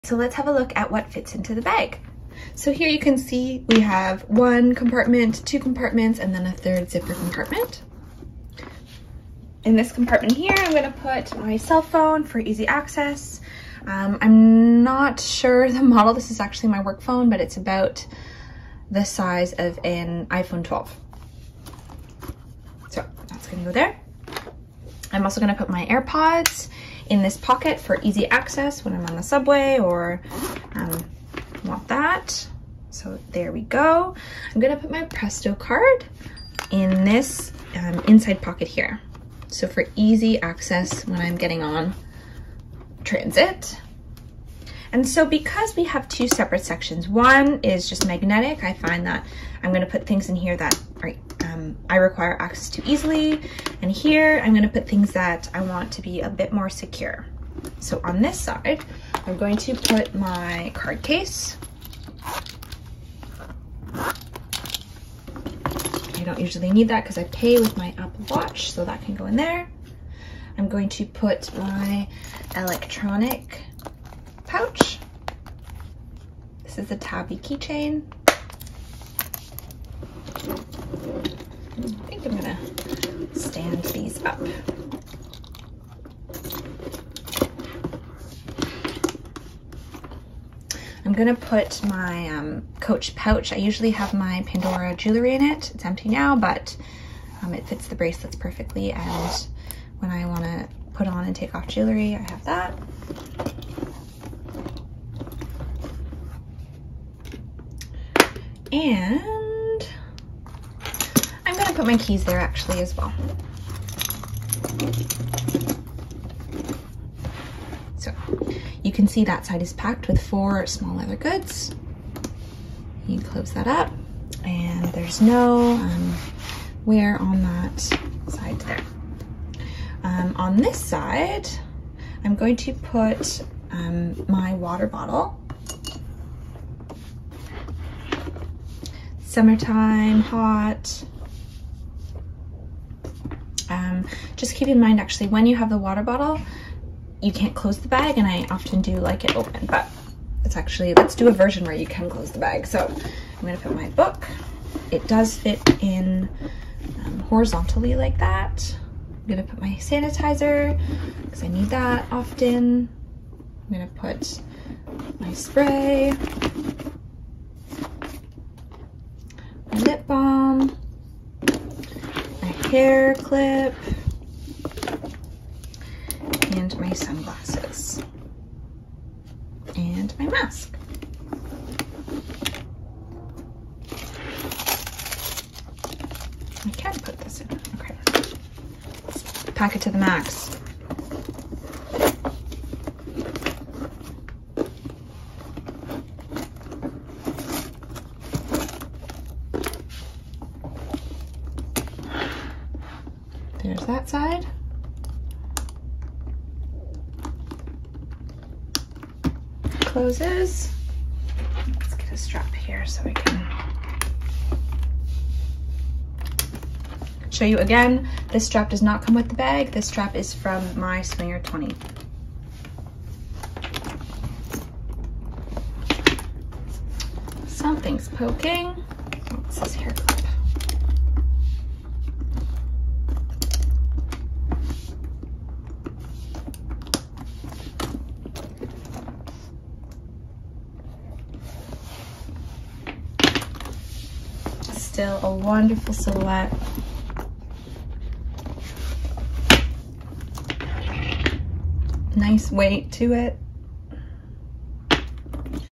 COACH WILLOW BUCKET BAG IN AZURE | PART 2: What Fits Inside | Terryberry
Hi All,
In this video, I show you how I pack my Coach Willow Bucket Bag in the colour Azure. The style is similar to the Coach Charlie Bucket Bag, so if you like that bag you may also be interested in this one! Keep watching if you like 'what's in my bag videos' and 'how I pack my bag' videos.
Kindly,
Taryn (Terryberry)